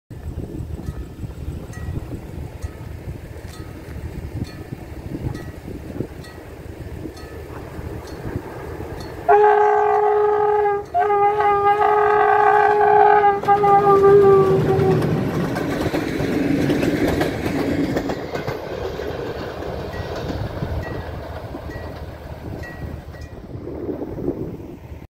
Trenul Regio Cluj Napoca - Sighişoara la Cojocna cu mecanic de nota 10
Super mecanic. Murdar trenul

Mulțumesc mecanicului pentru salut